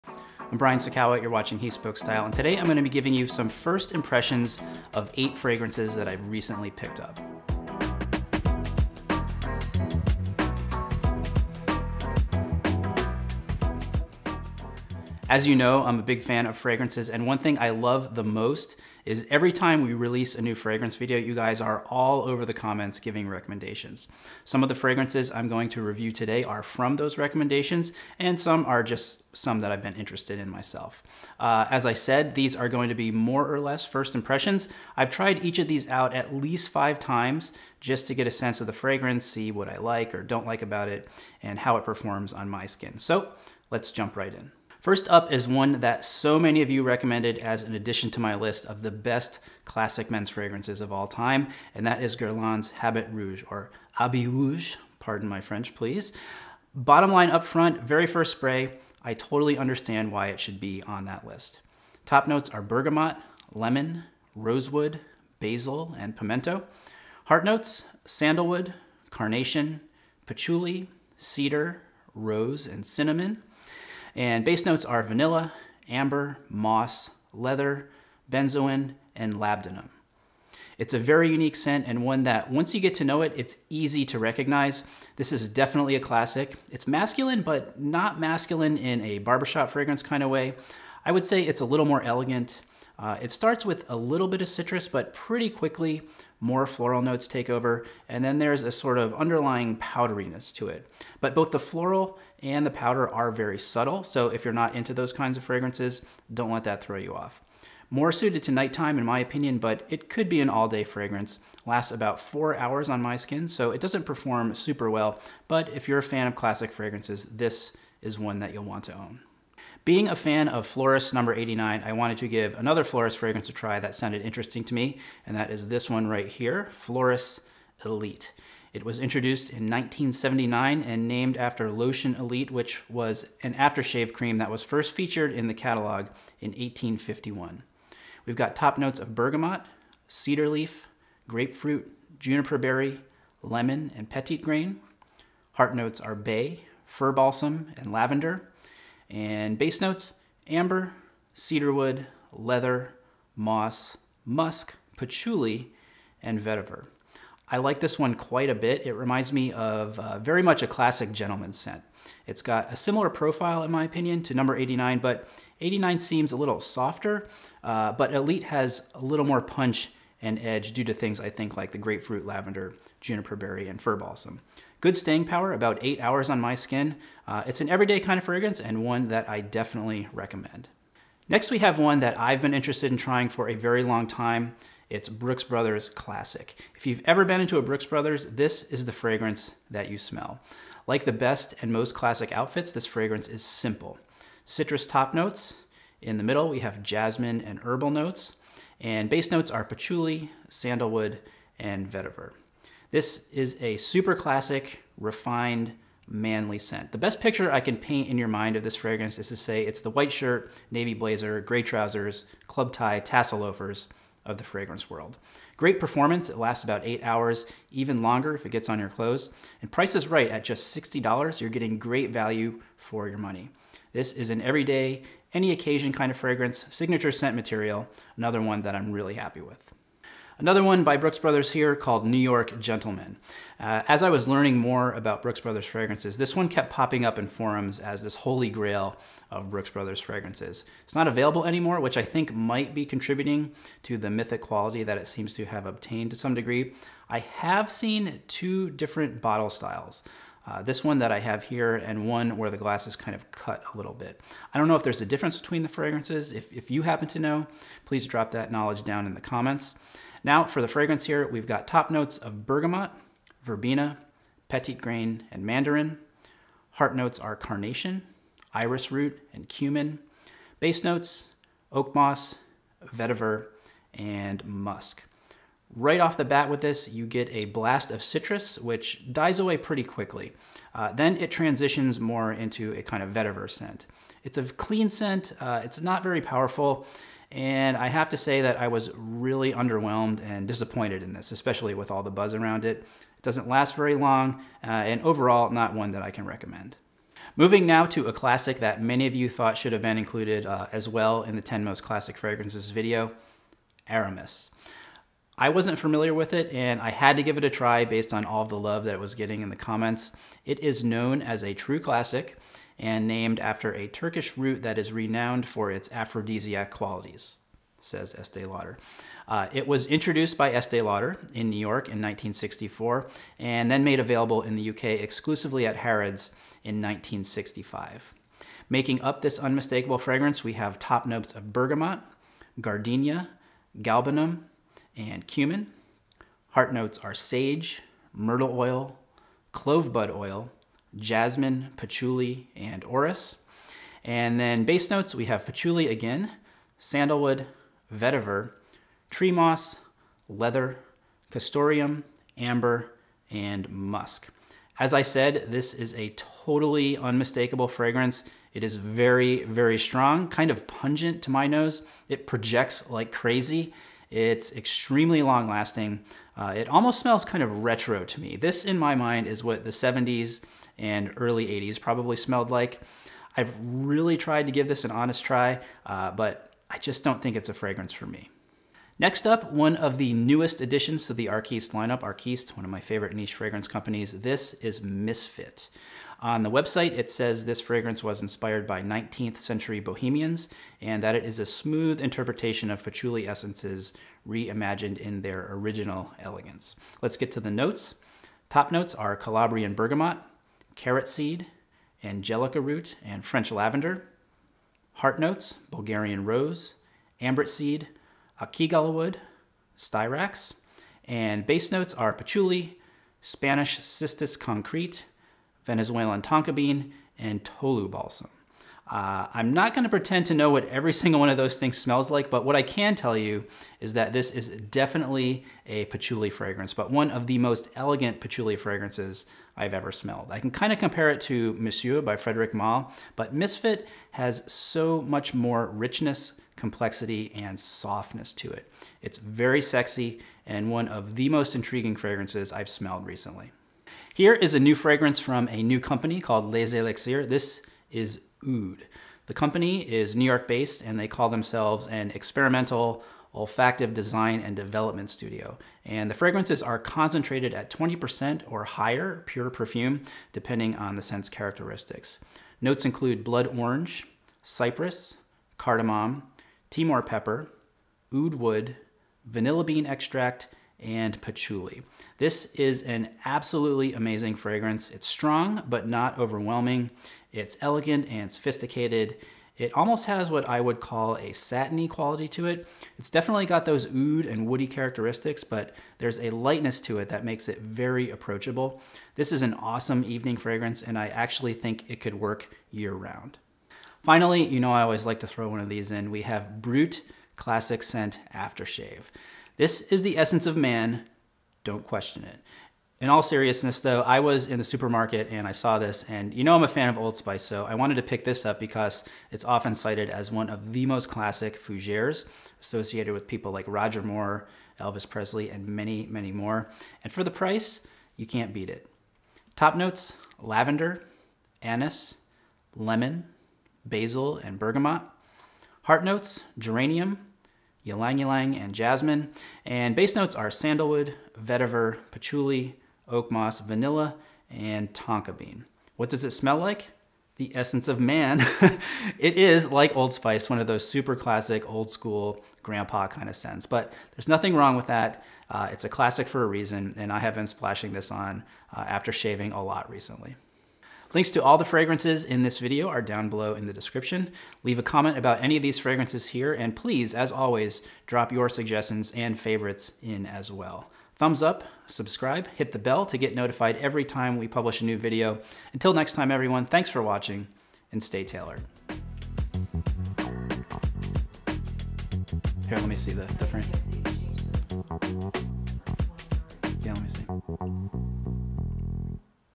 Men's Fragrance Haul & Review | Habit Rouge, Aramis, Arquiste, Les Élixirs, Brooks Brothers
You know that I'm always on the hunt for the best men's fragrances and I love all the recommendations I get from you every time we publish a new fragrance review video. In today's video, I'm taking you through eight of my most recent men's fragrance acquisitions and giving you some first impressions. It's a mix of classic fragrances, niche fragrances, and some of the newest out there. Enjoy!

0:00 - New Fragrance Haul
0:45 - Guerlain Habit Rouge
2:00 - Floris Elite
3:01 - Brooks Brothers Classic
3:50 - Brooks Brothers New York Gentleman
4:57 - Aramis
6:18 - Arquiste Misfit
7:24 - Les Élixirs Oud
8:14 - Brut Classic Scent After Shave